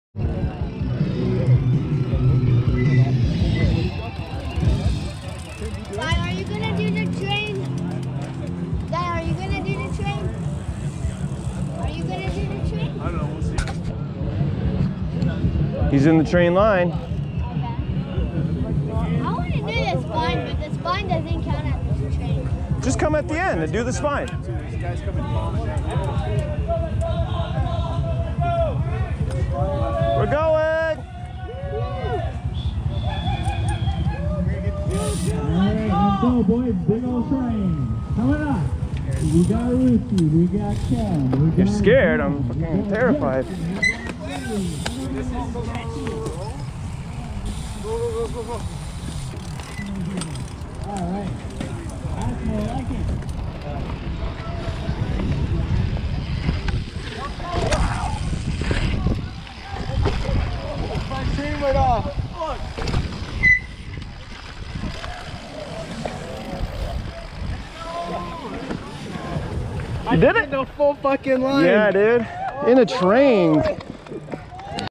Woodsbmx Train Line
We headed out to the west island to ride the annual Woodsbmx jam!
They asked everyone that had cleared the big line of jumps to ride them in a train together, so we all got it done!
What a fun day, now I'm gonna go and sleep for a month!